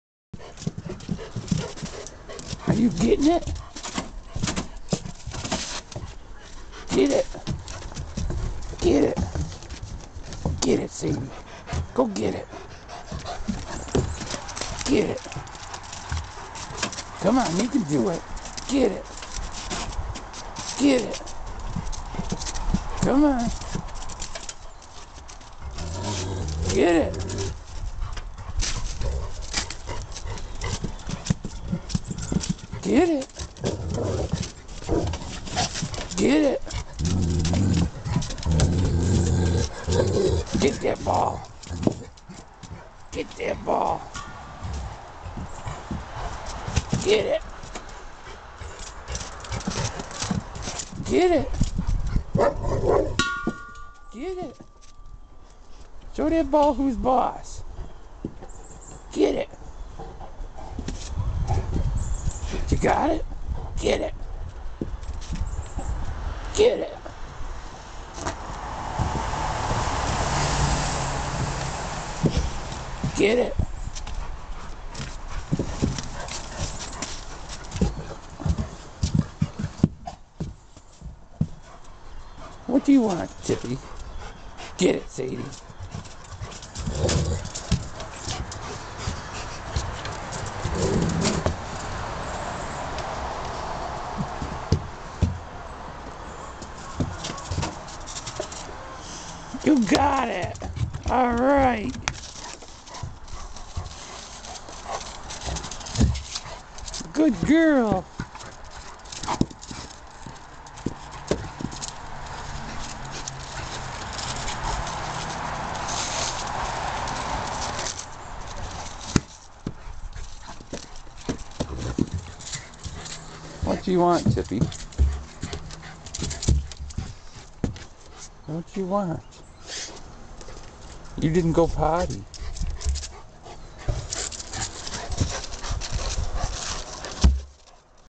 sadie 1
A short clip of Sadie and her favorite toy, a 16 pound bowling ball, with Tippy looking on. She would never let him play with any of her bowling balls. She had 3.